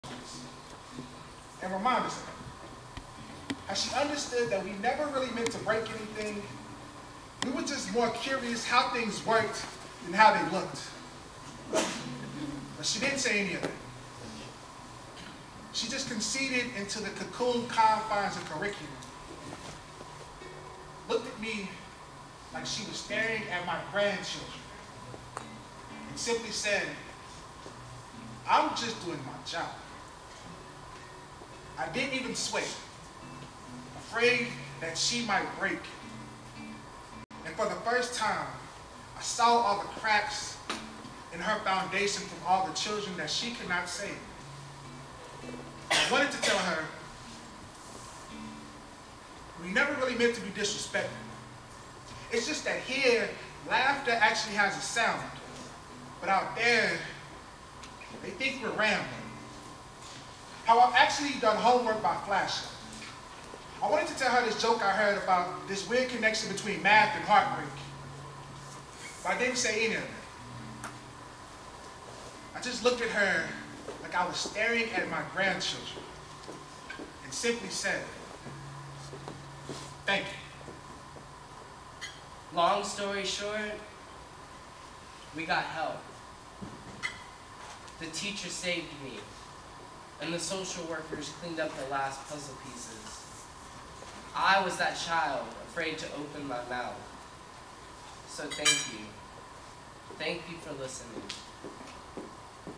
NAREN ENGAGEMENT PART 8 - TEACHER
Justin Bullard and Willie Holmes pay homage to that certain teacher.

Authoring Action Assegai Ensemble presented to 235 Educators, Administrators, Counselors as Keynote at the National At Risk Educators Network Conference Luncheon, April 24, 2014.  The theme was Transforming Education: Transforming Ourselves.

The next day, Artistic Director, Nathan Ross Freeman and Assegai Authors conducted a 3 hour workshop on Authoring Action's writing and presenting process.

NAREN awarded Authoring Action a Partnership for the 2015 conference and we are now an annual keynote presenter and workshop